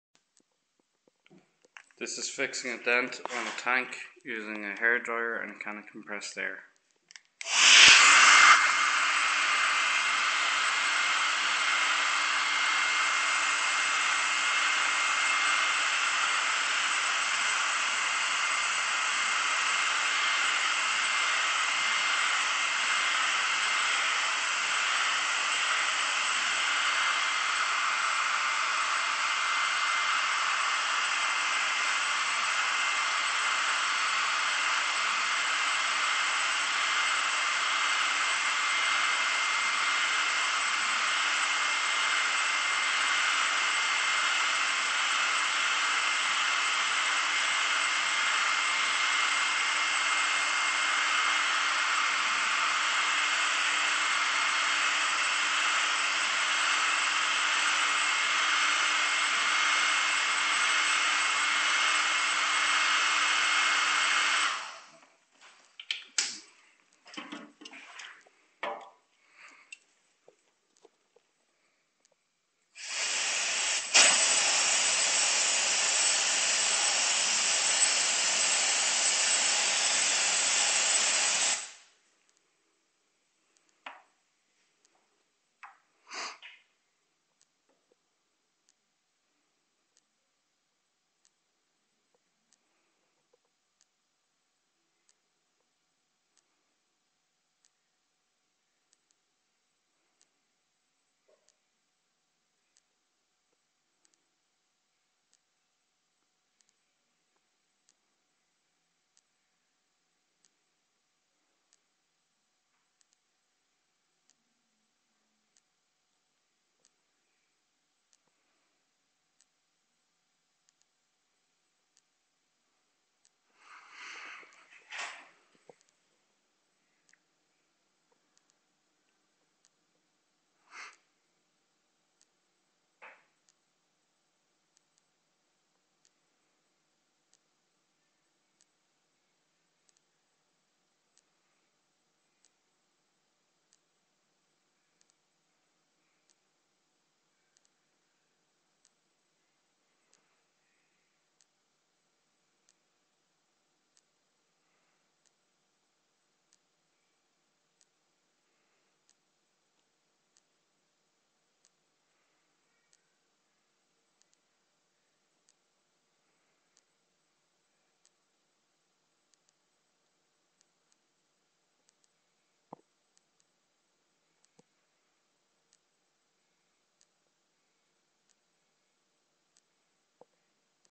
Testing a trick to see if it can pop out a dent on a tank
Sadly it didn't work.
Your meant to heat up the dent and then rapidly cool it. Done with a hair dryer and a inverted can of compressed air. It is meant to work on car doors and panels.